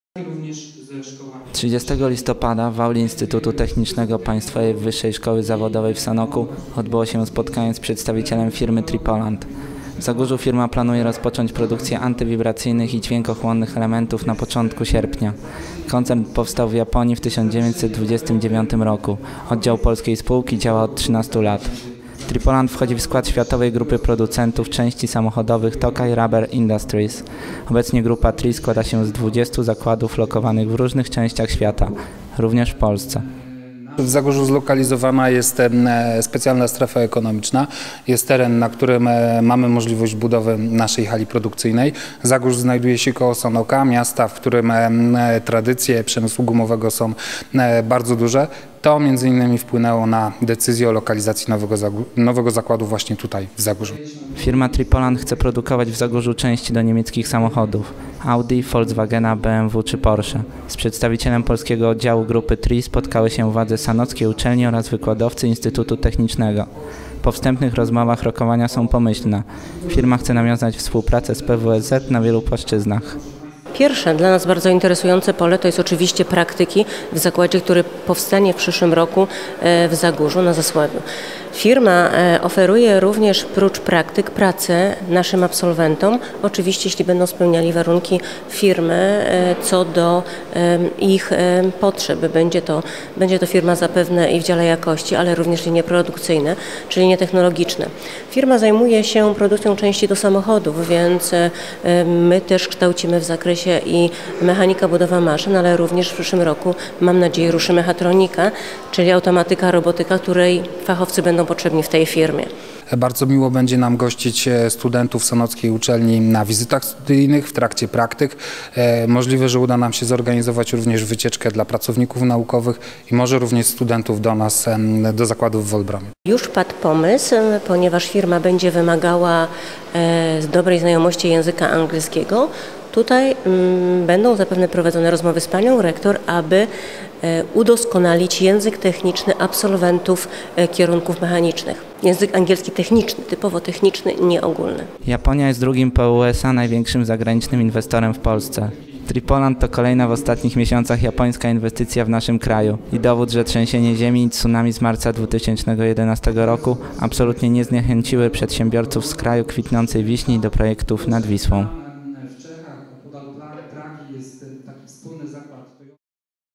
TRI Poland chce współpracować z PWSZ Sanok
TRI Poland, producent japońskich części samochodowych, chce nawiązać współpracę z PWSZ im. Jana Grodka w Sanoku. Firma buduje właśnie swoją fabrykę w Zagórzu i już szuka pierwszych pracowników - także wśród naszych studentów.